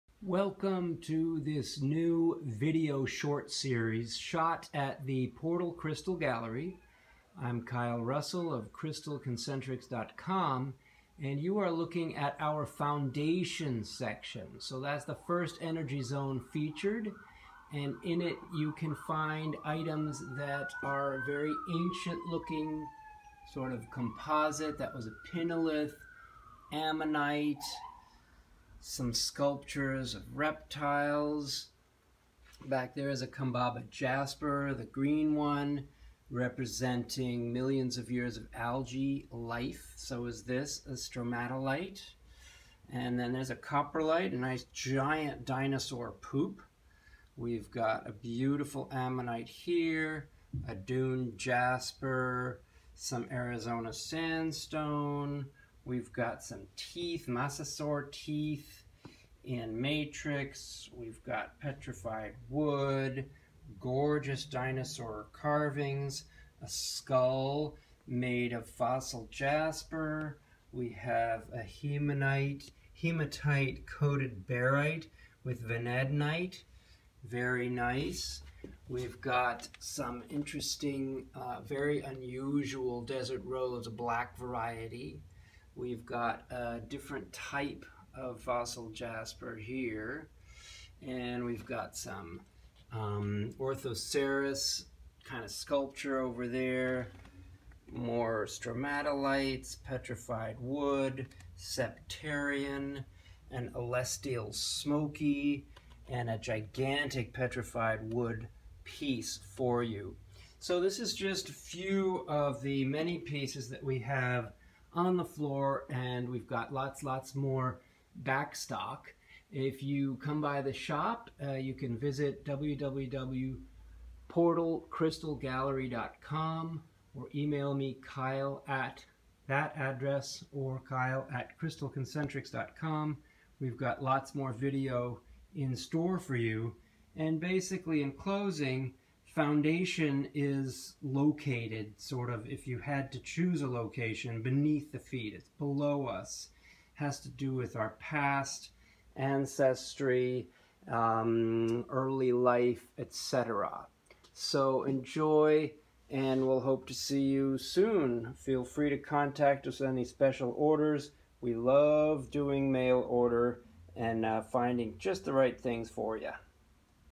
Crystal Tour - Foundation Energy Zone - Portal Crystal Gallery
Each of the many sections of the Portal Crystal Gallery deserve highlighting - in this case, the first or Foundation section. It's "Locus" (where it's located, vis a vis the Chakra system) is below the feet. And it refers to our past, in this and previous lives, as well as our families of origin (and ancestors). If you see something you like, or would like to see more examples of any of these items, write . We love Mail Order, and would be honored to find something interesting for you to add to your collection!